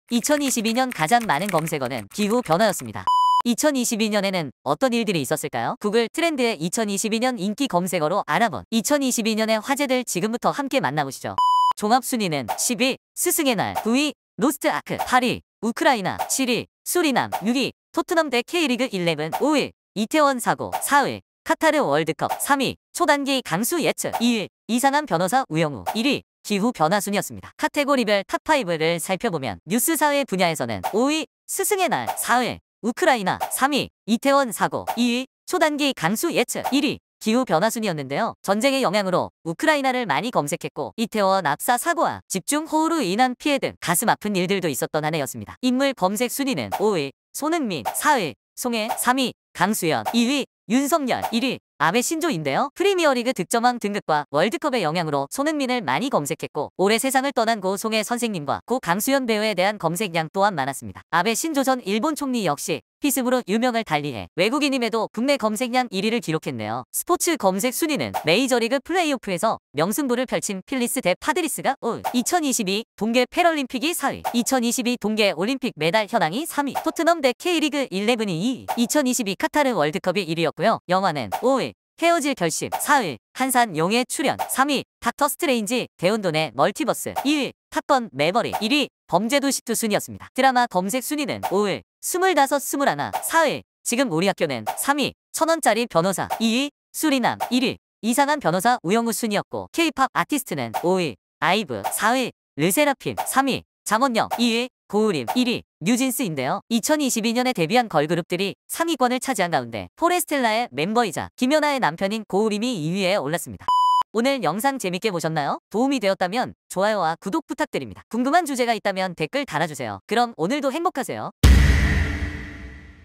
2022년 한국에서 가장 뜨거웠던 ‘구글 검색어’ 1위는?
안녕하세요 :D
오늘의 5초 주제는 "2022년 한국에서 가장 뜨거웠던 구글 검색어" 입니다.